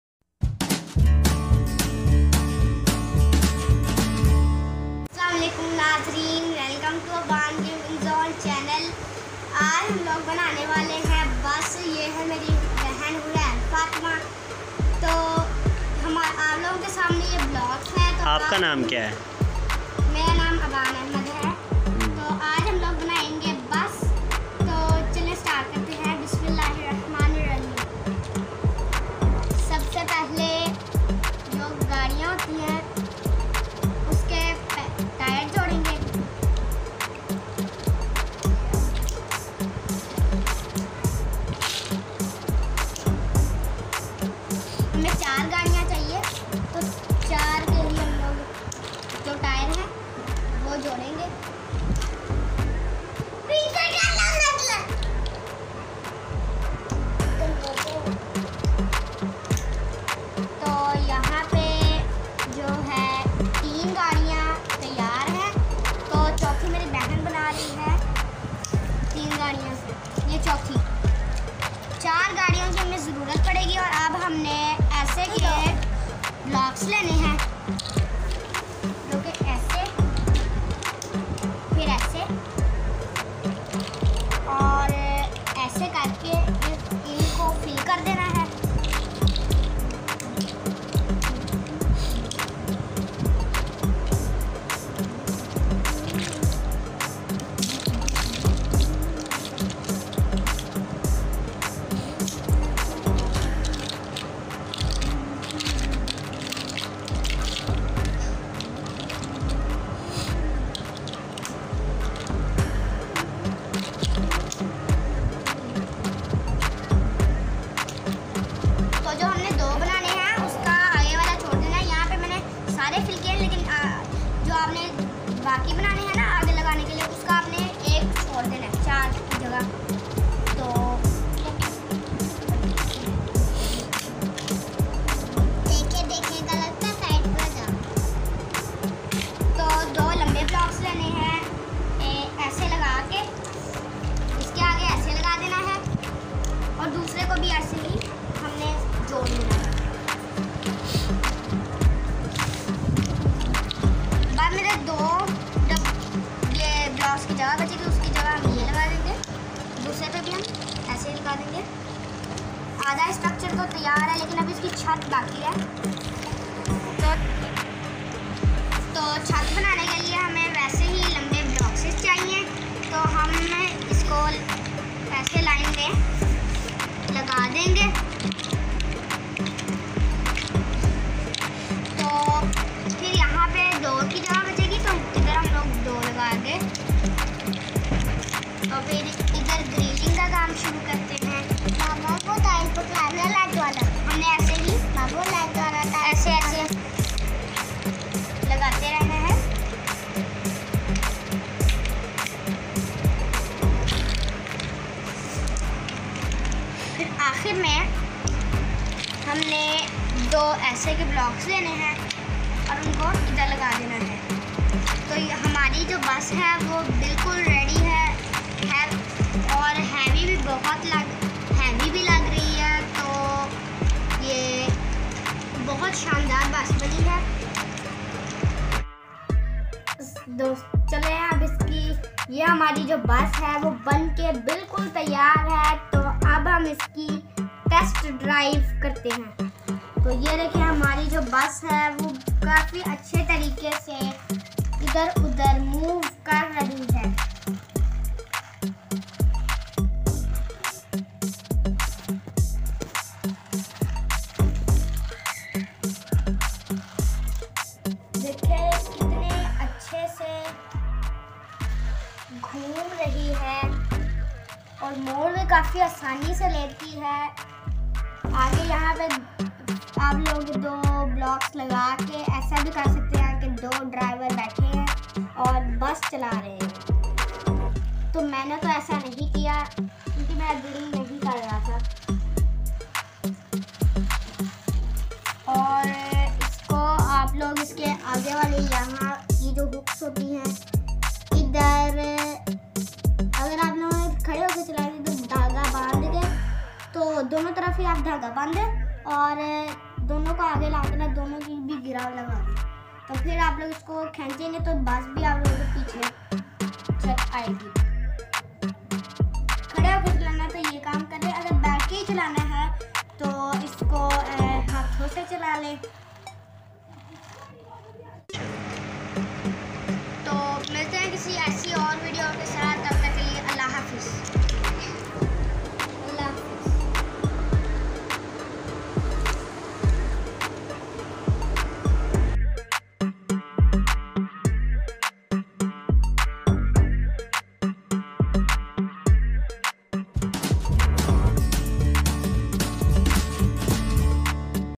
Making a bus with Blocks || How to Play with blocks || Blocks Games
This is our 1st proper video of my channel. In this video We will make a bus with color toys. Hope you will enjoy.